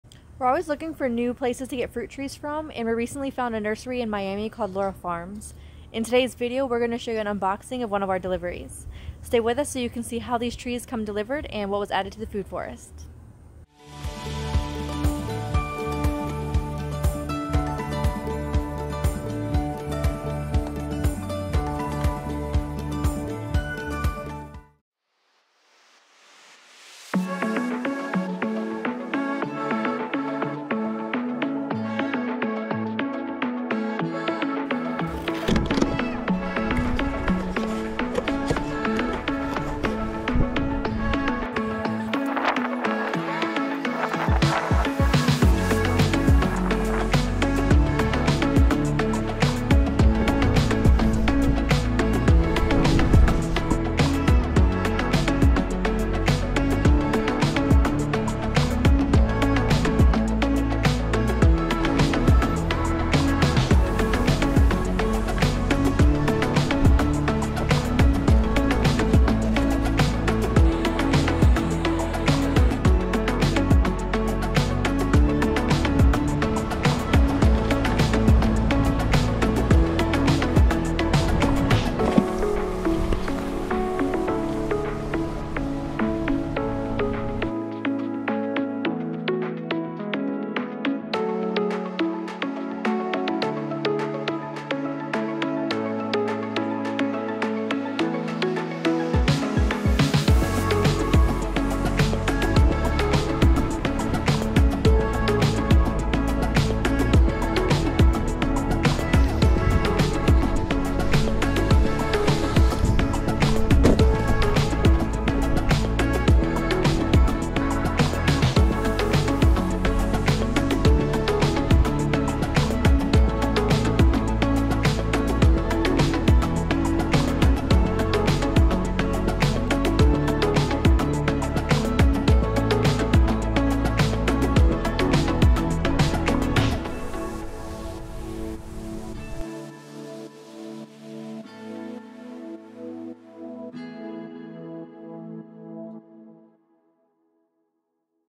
Unboxing FRUIT TREES from Lara Farms
We recently received a shipment of new Fruit Trees from Lara Farms! Check out what it's like to order trees online and see what was added!